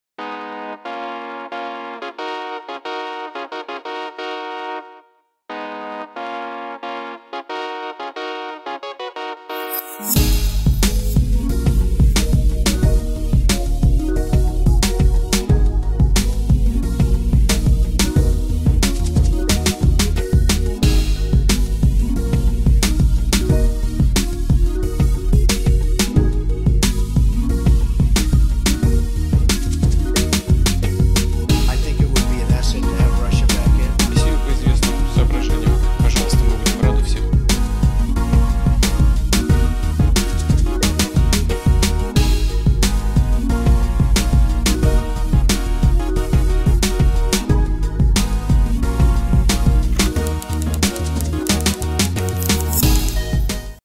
Trump-Putin Summit: What To Expect
Donald Trump and Vladimir Putin will hold their first one-to-one summit on July 16 in Helsinki.